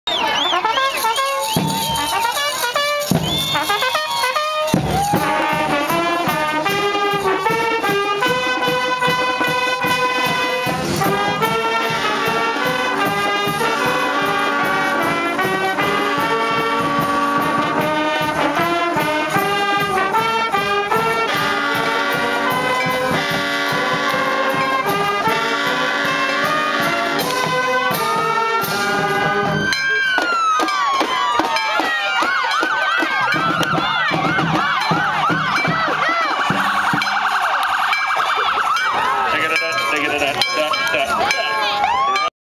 Idalou Wildcats vs. Abernathy Antelopes 2009
Idalou's 100th win of the decade, they defeated Abernathy 30 - 21 on October 30, 2009...this was also the senior night.